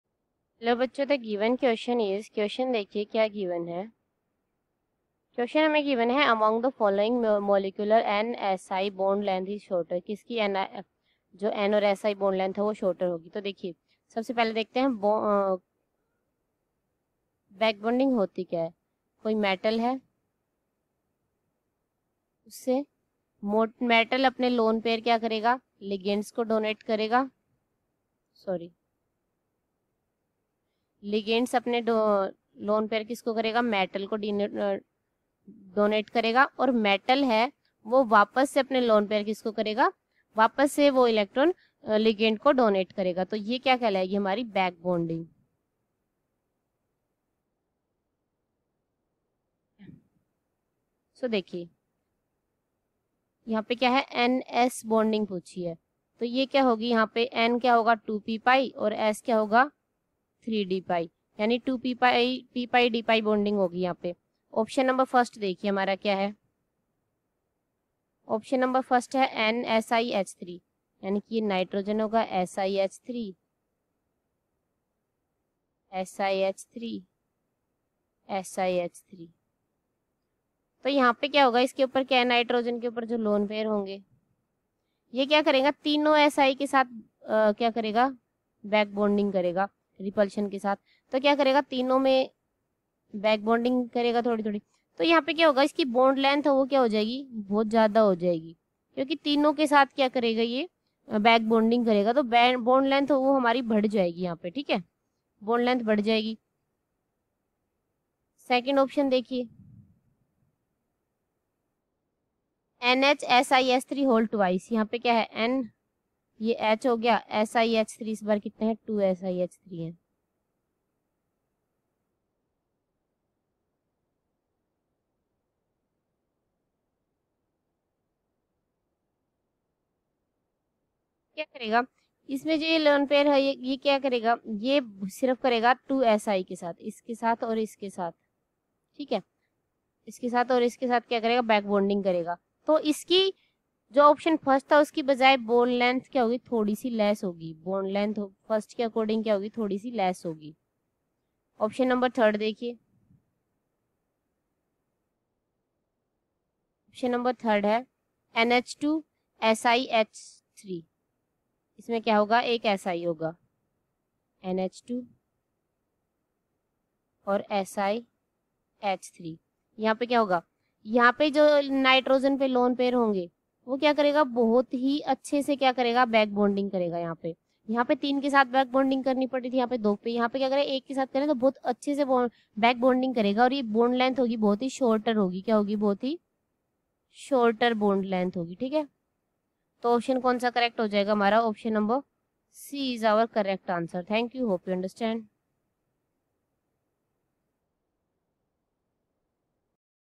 Among the following molecules \( \mathrm{N}-\mathrm{Si} \) bond len...
Among the following molecules \( \mathrm{N}-\mathrm{Si} \) bond length is shortest in :
\( \mathrm{P} \)
(a) \( \mathrm{N}\left(\mathrm{SiH}_{3}\right)_{3} \)
(b) \( \mathrm{NH}\left(\mathrm{SiH}_{3}\right)_{2} \)
W
(d) All have equal N-Si bond length